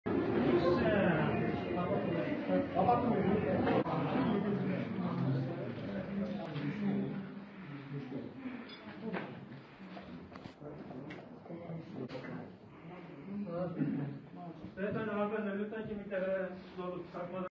İçkili eğlenceye jandarma baskını
Elazığ'da koronavirüs tedbirlerini ihlal ederek restoranın camlarını kalın perdelerle kapatıp, içkili eğlence yapanlar jandarma baskını ile yakalandı.